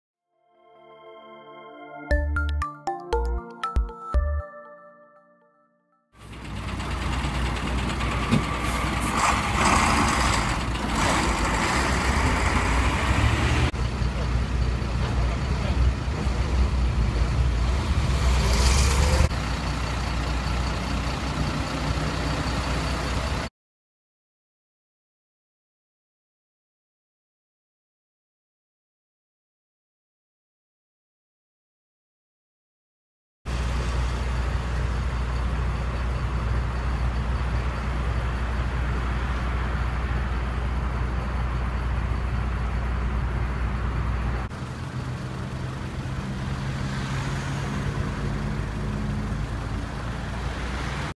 Тула. Лобовое столкновение на ул. Рязанской. 27.07.2014
Предварительная причина ДТП - водитель автомобиля Kia уснул за рулем. Пострадал предполагаемый виновник столкновения, а также водитель и пассажирка отечественной легковушки.